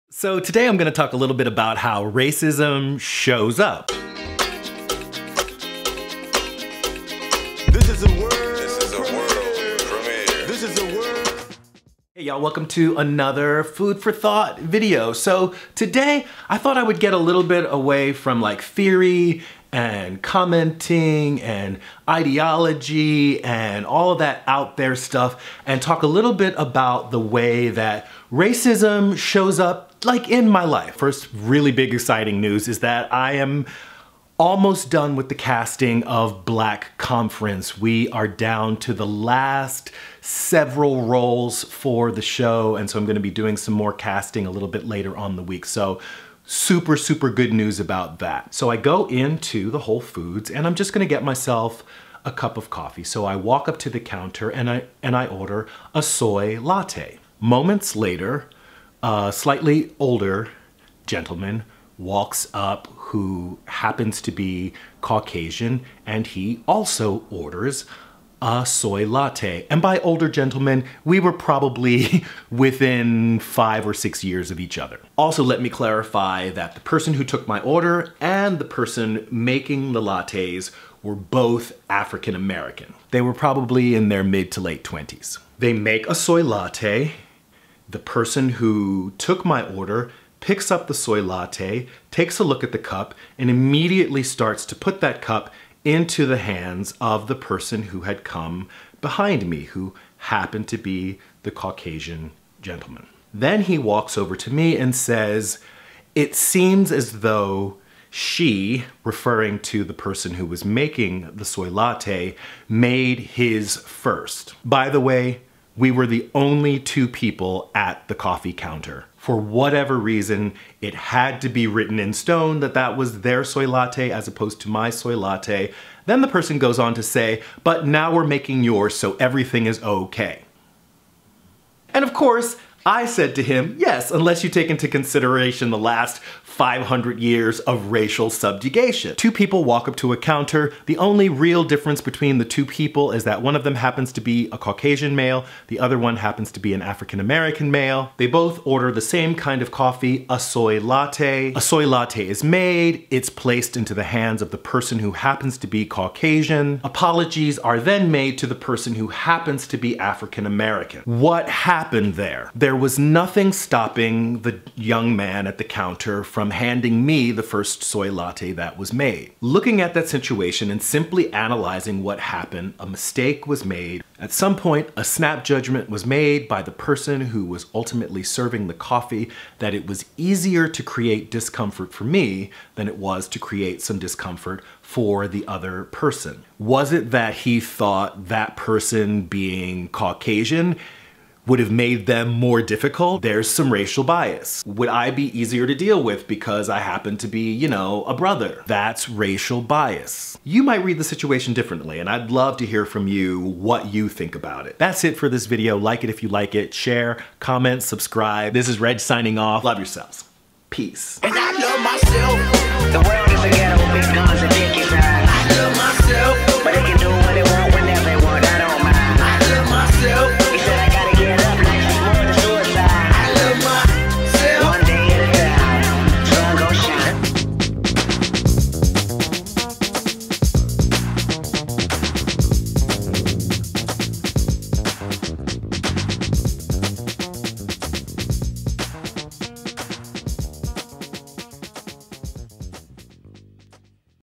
How R cism Shows Up In My Life
This is a little story-time moment on something I experience fairly regularly. Is it how racism might be showing up in my life? I will leave it for you to decide.

Camera: Canon EOS 70D
Lens: Cannon EF-S 10-18mm f/4.5-5.6 IS STM
Mic: RODE VideoMic Pro
Tripod: Manfrotto classic - mini ball head w/ quick release